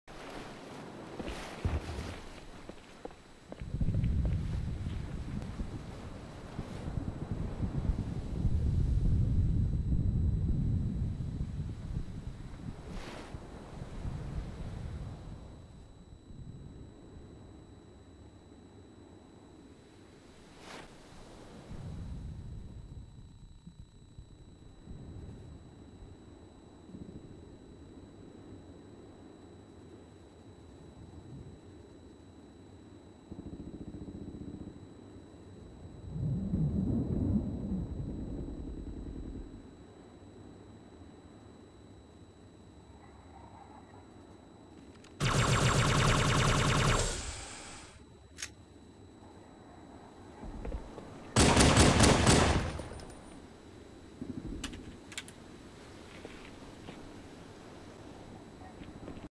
PT wet slant.avi
the only clip i managed to save after getting a virus on my pc.  this was some footage of a bounce montage i was going to make a few years ago.